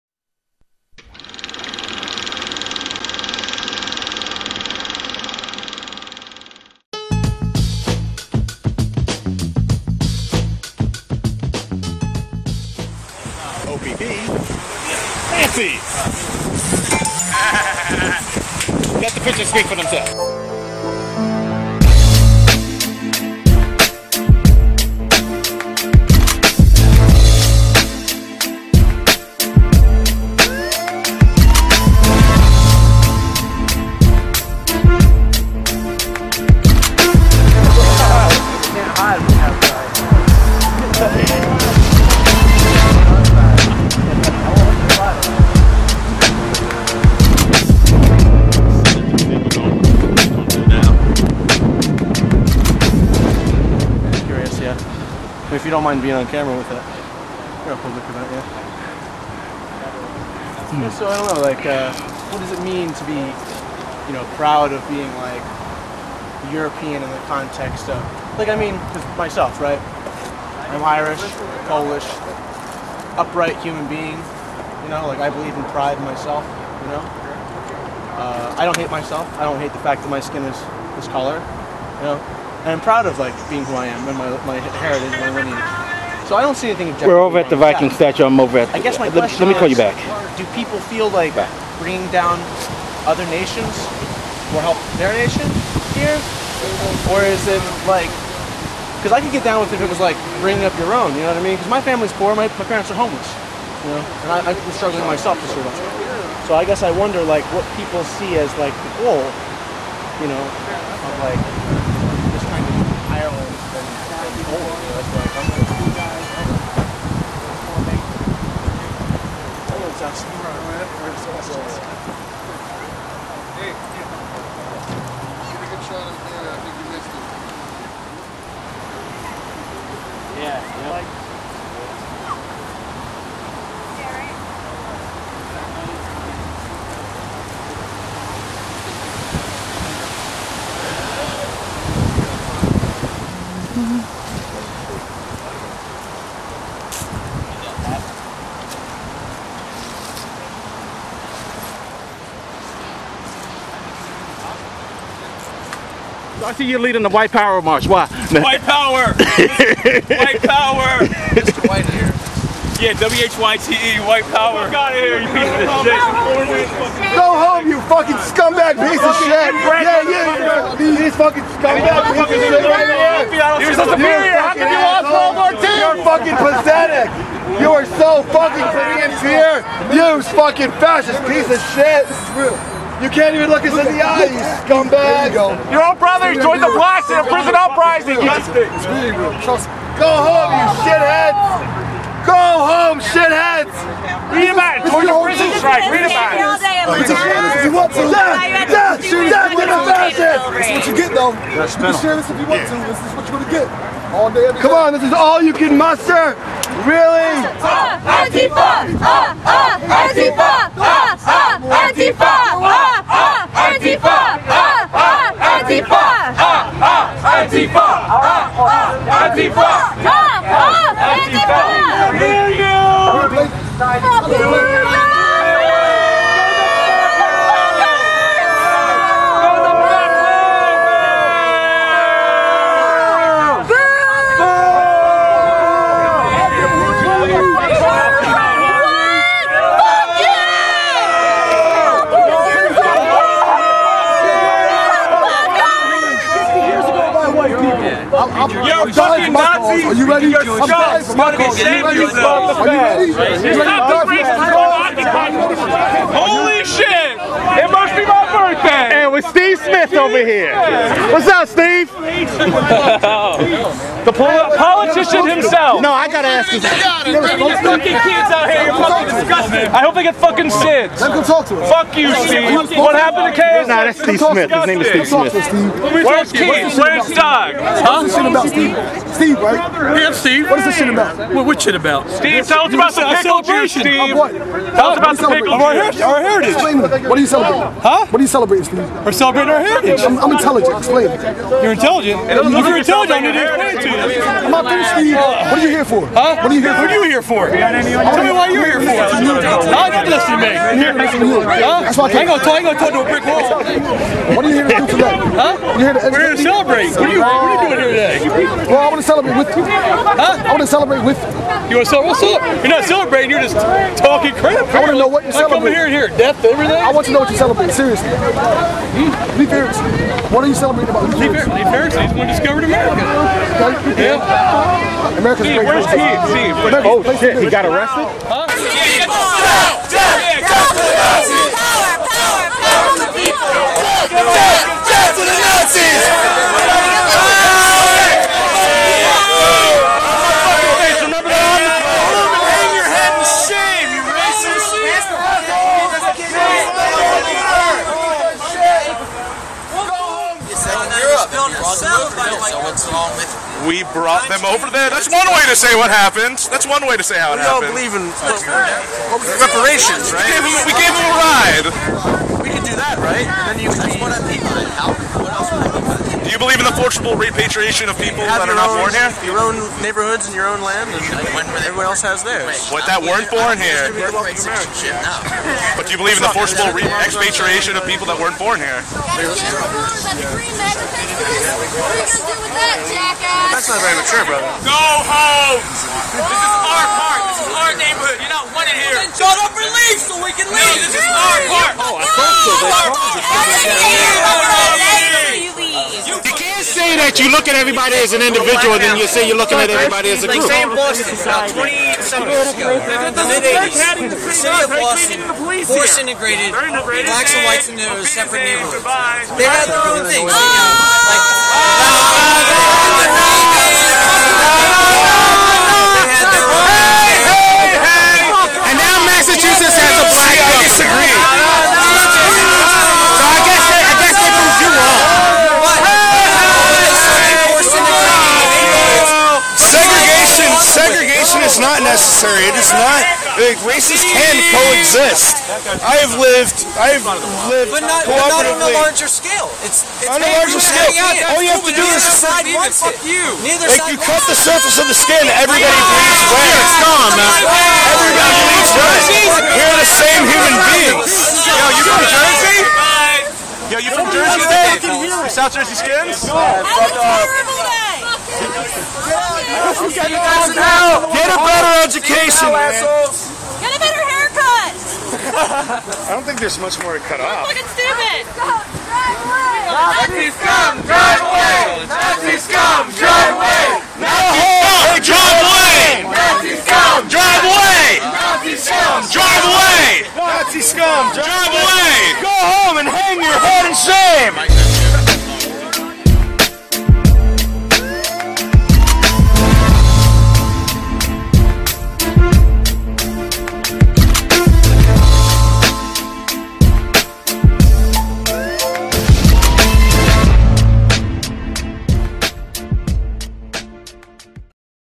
Occupy Boathouse Row! How Antifa Ruined A Nazi Outing in Philadelphia
Leif Ericson Day is Oct. 9, which is also around the weekend that Columbus Day is observed. Since 2007 on or around the Columbus Day Weekend, neo-Nazis come out to Philadelphia's Boathouse Row to hold a "Leif Ericson Day Celebration" at the "Viking Statue" - a statue of Viking explorer Thorfinn  Karlsefni. Little press is given to the event, but opposition has been growing more and more with each passing year. However, the Nazis have been losing numbers, especially since their sponsoring organization the Keystone State "Skinheads" has been in disarray since their last appearance in 2009 - which just so happened to be that years Leif Ericson outing and the year they saw their first protest. In 2011, they returned courtesy of organizing by bonehead Anthony James (AJ) Olsen (the one in the video with the megaphone), but antifa came from all over the area, some from as far as Portland to oppose them this year. There was one arrest - on the Nazi side (a minor), and the Nazis were so paranoid that they drove their cars backwards to leave so no one would see their rear license plates. This is video from that event